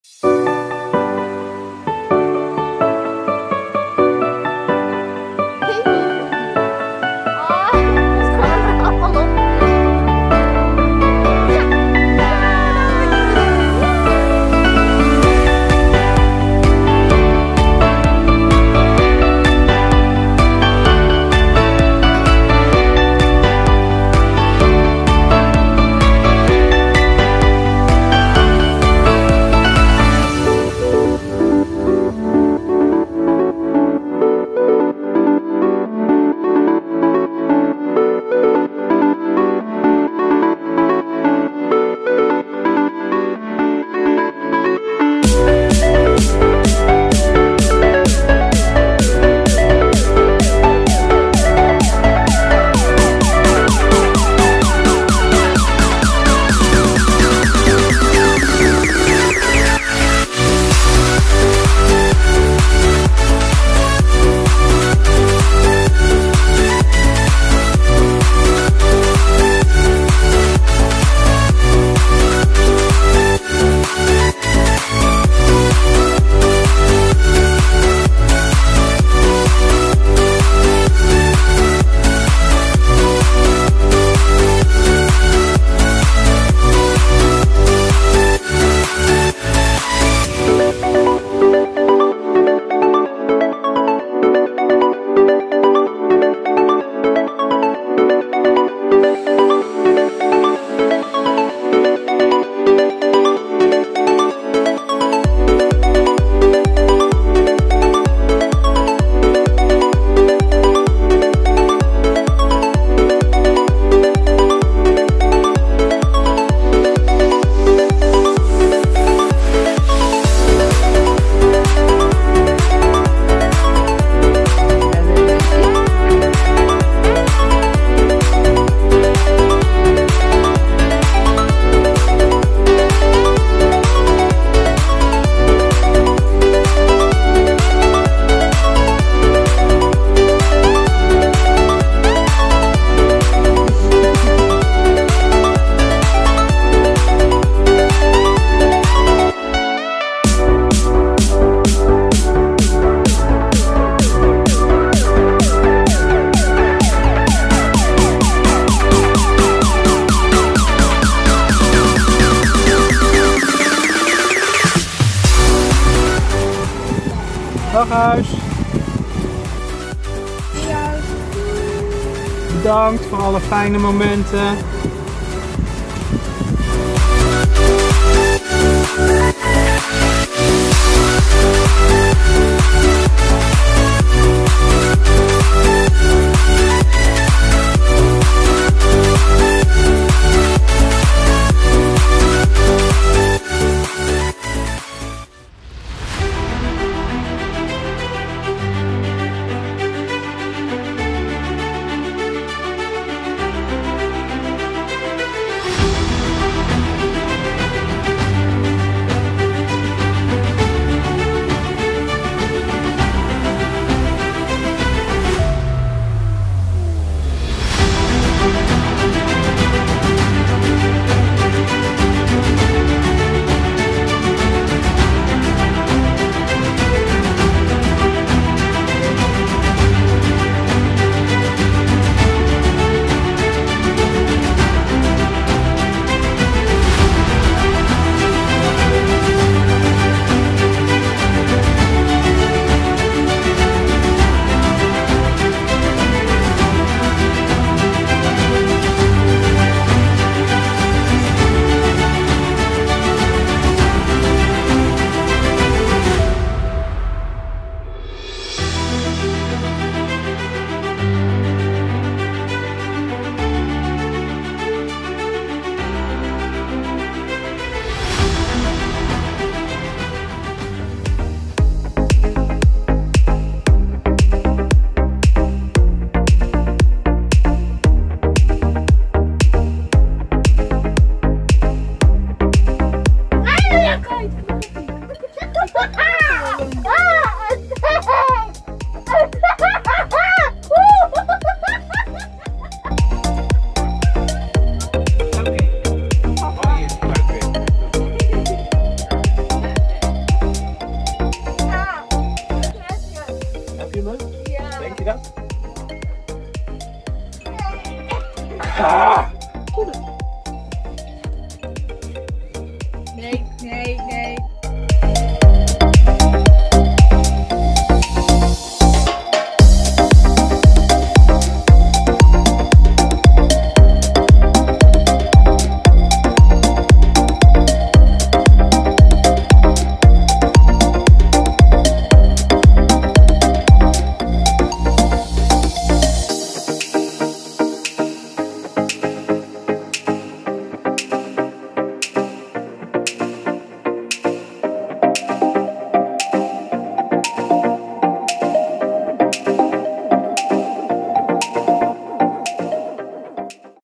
TIMELAPSE | In een vogelvlucht van woning naar caravan
Korte TIMELAPSE video rondom de verkoop en verhuizing van onze woning naar ons tussenstation: de caravan. In de komende afleveringen zullen we jullie stapsgewijs hierin mee nemen, maar omdat juni nog zo lang duurt ;-) bij deze alvast een kleine impressie van wat jullie kunnen verwachten.